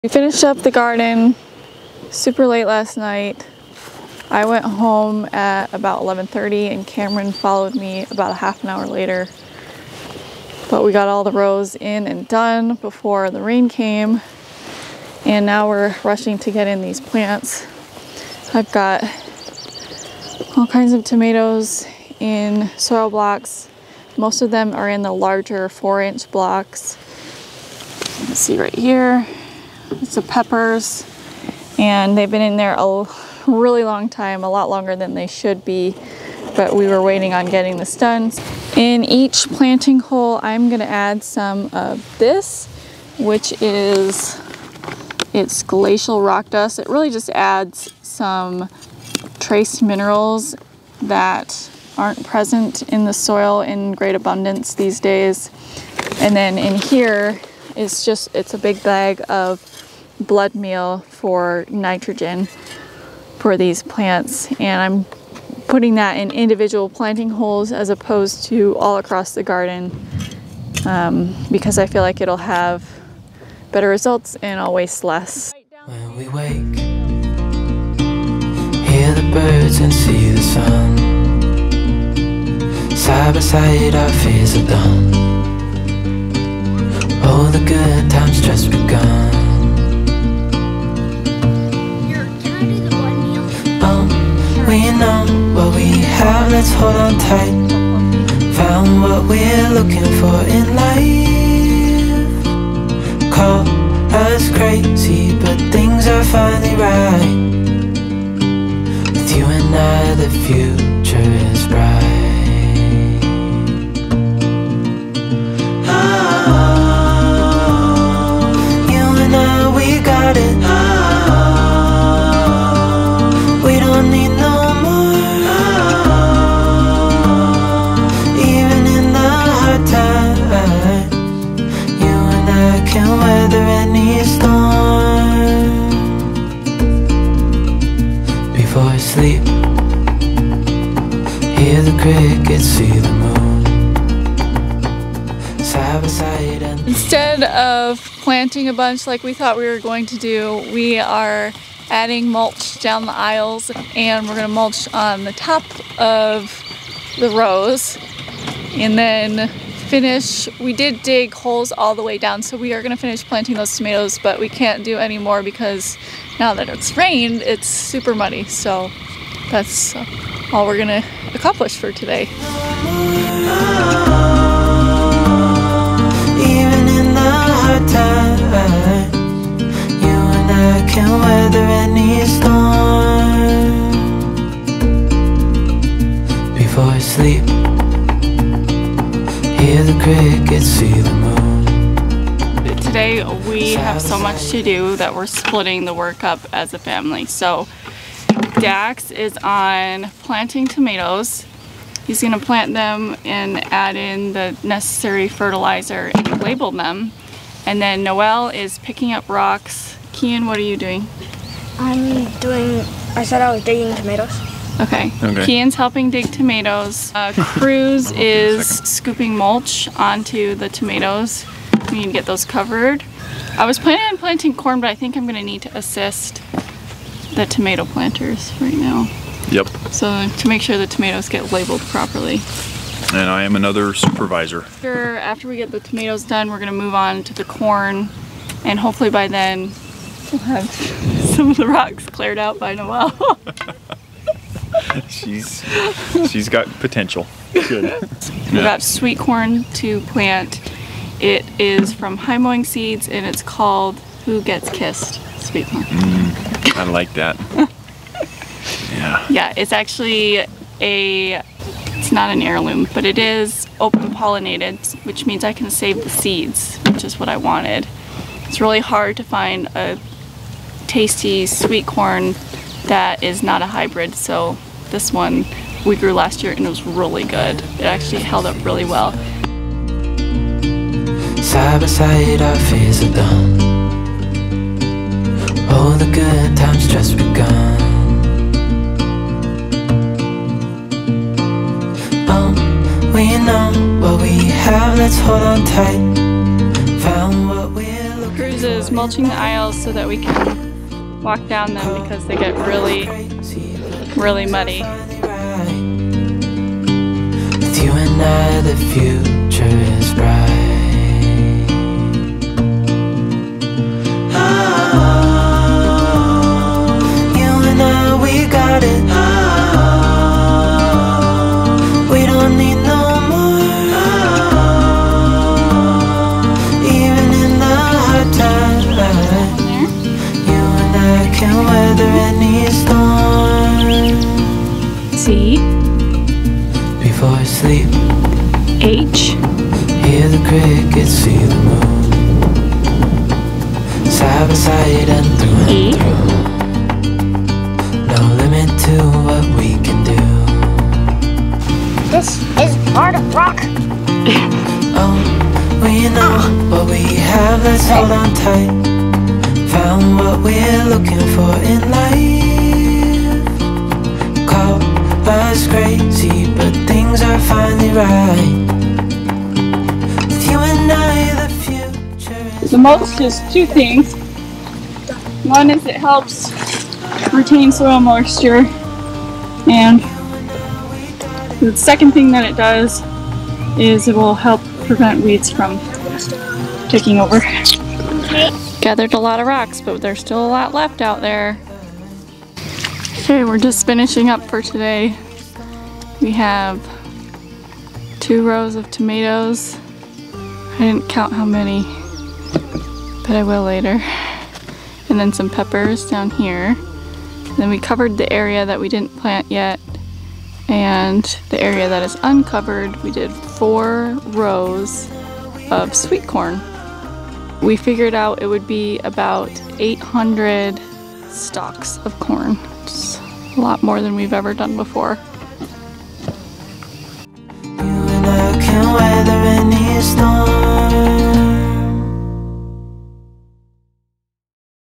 Planting a Year's Supply of Tomatoes and Corn (VLOG)
Working together as a family to plant a year's supply of tomatoes and corn. Rain or shine!